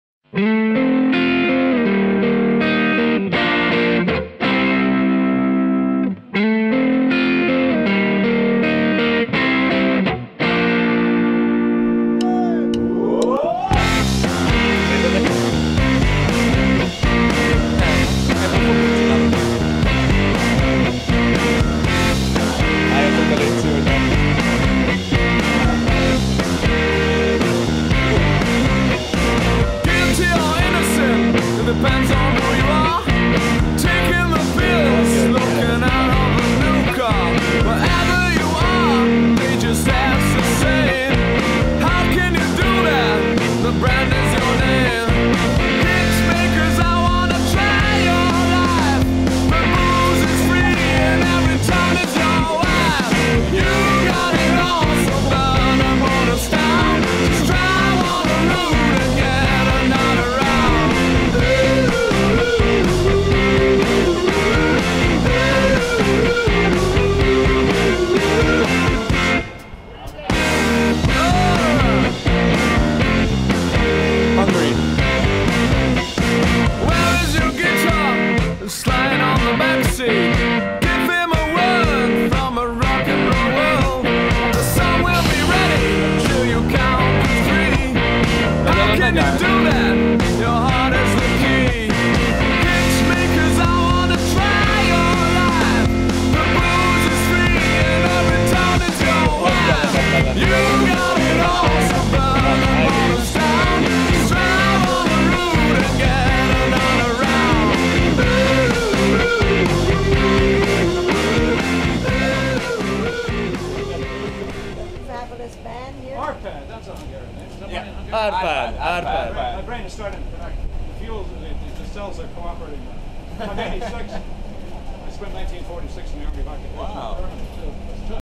New York-i turnéról üzen az Ivan & The Parazol
Kéthetes New York-i turnéra indult nemrég az Ivan & The Parazol. Nagyon zenész barátaink videoklipben üzennek a Petőfi Rádió hallgatóinak arról, milyen ma a rock 'n' roll!